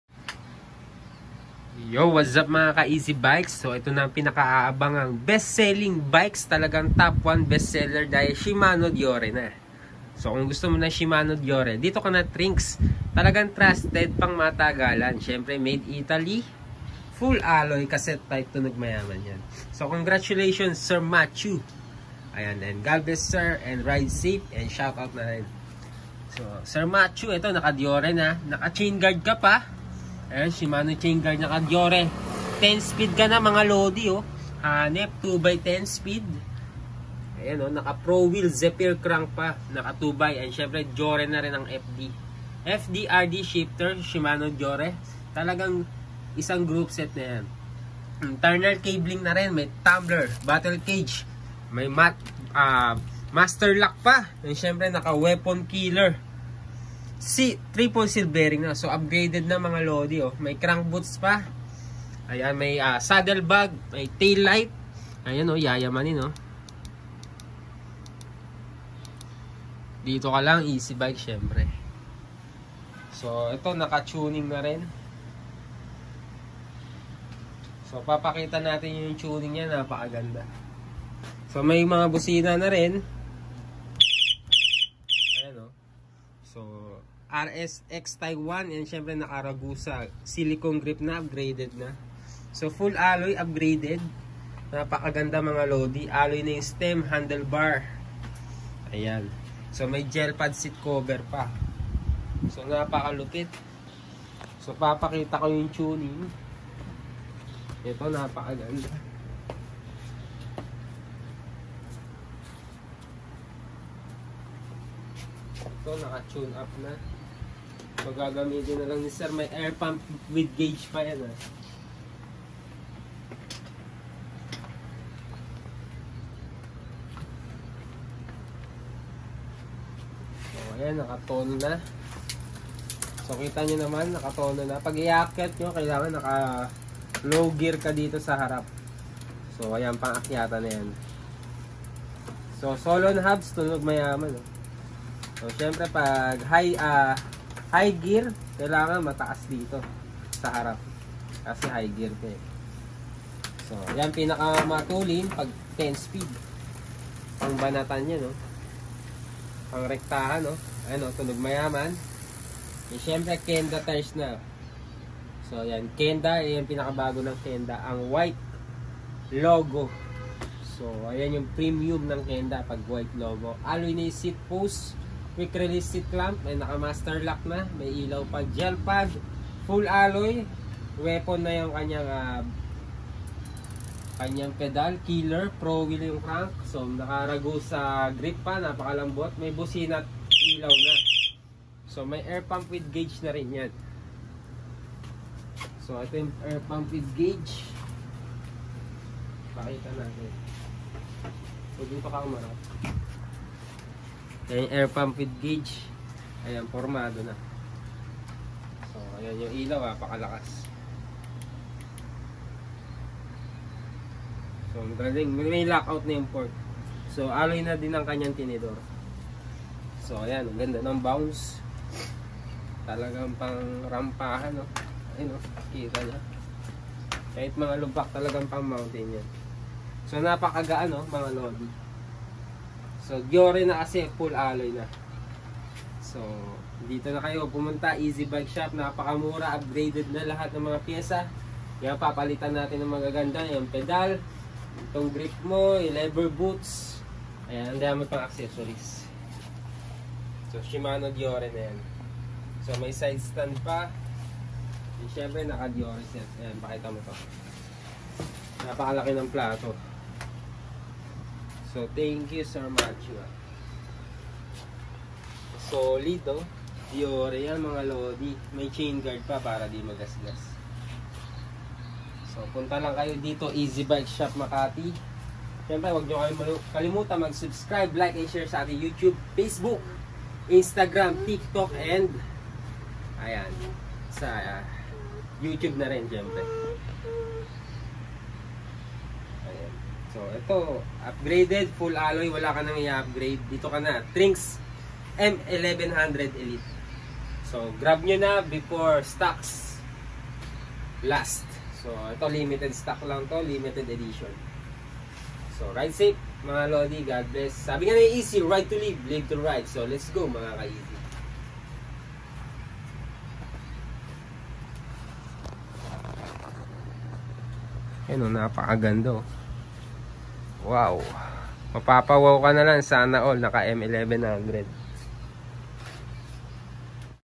Trinx m1100 elite 10speed Deore
Trinx M1100 Elite 2022 Model Latest Version
Upgraded Weapon Savage Triple Sealed pedal
27er Hydraulic 2x10 Shimano Deore Groupset Full Alloy

Specs
Alloy Frame
Weappon savage triple sealed pedal alloy
Ragusa handle Grip
Saddle Bag
Lever boots
Crank boots
Chainguard
Tailight
Frontlight
Internal Cabling
Alloy Fork
Medium Frame
27x2.10 Tires Kenda
2x10 Shimano Deore Groupset
Lockout fork
Hydraulic brakes
Alloy Handle Bar
Alloy Seatpost
Alloy stem
Alloy Pedals
Alloy Crank
Alloy Hubs Solon Tunog Mayaman
Quick release tires
Sealed bearing bottom bracket ready for long Drive

Gcash Name
Christian Adam B. Flores

Waze: EazyBikes

5512 Dm Rivera street poblacion makati city